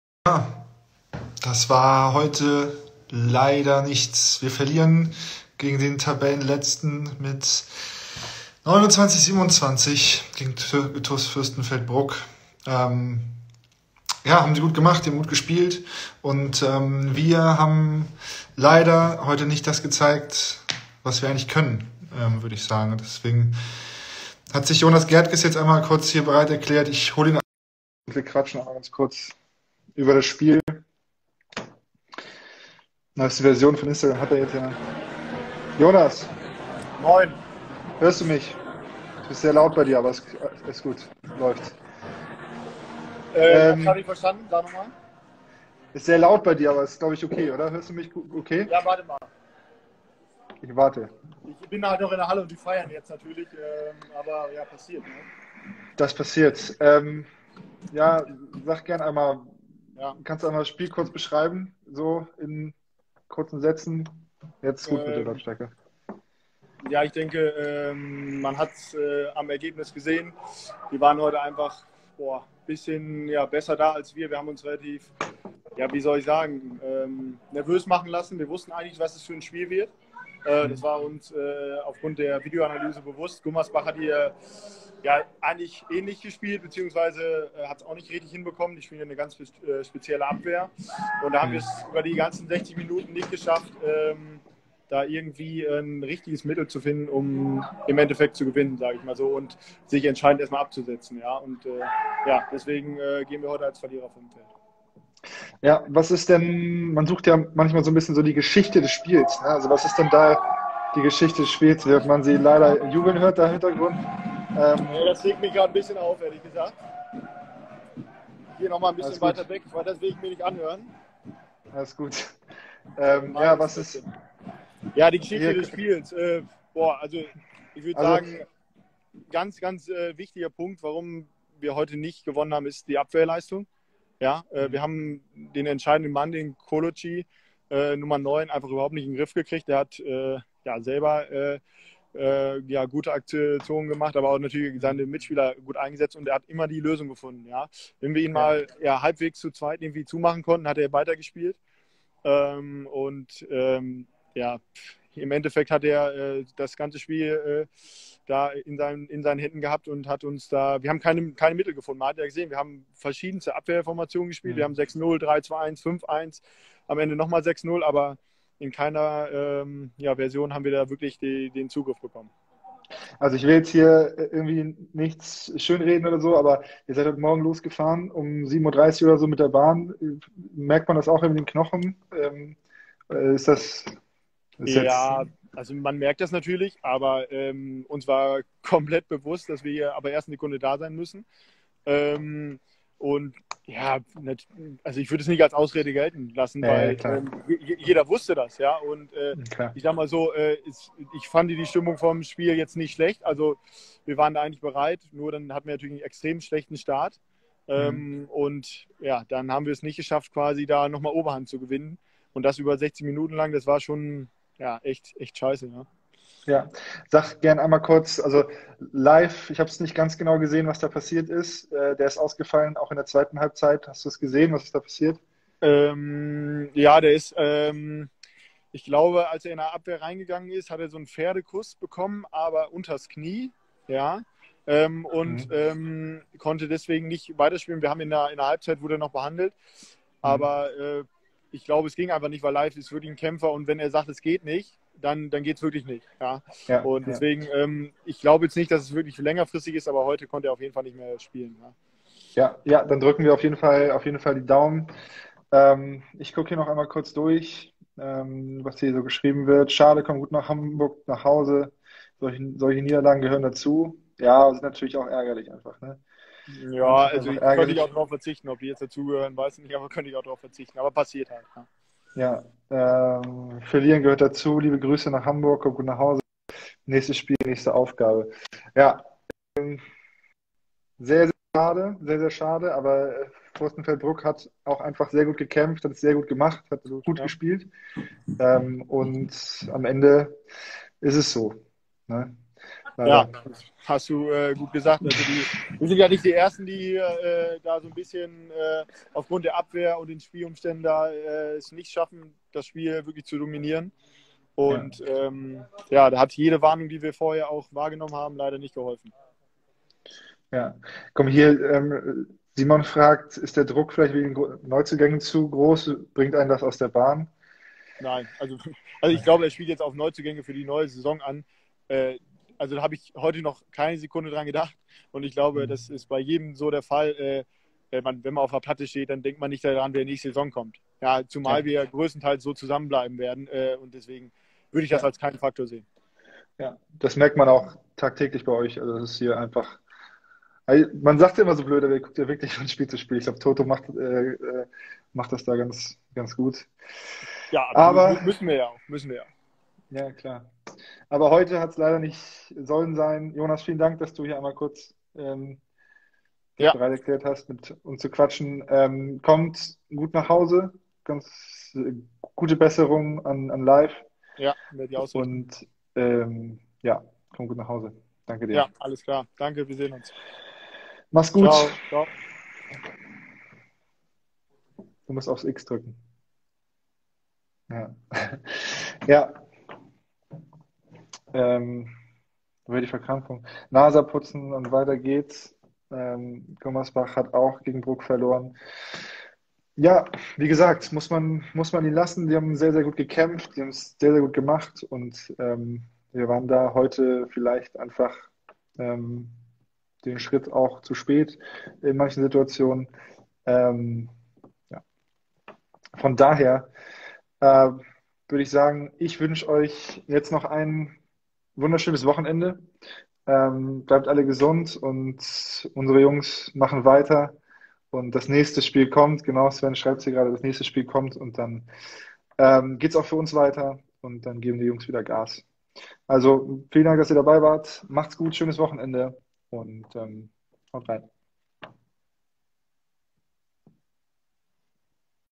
"HEJ, wie war das Spiel?" | TuS Fürstenfeldbruck
Nach der Auswärtsniederlage beim TuS Fürstenfeldbruck hat Jari mit Jonas Gertges kurz das Spiel analysiert.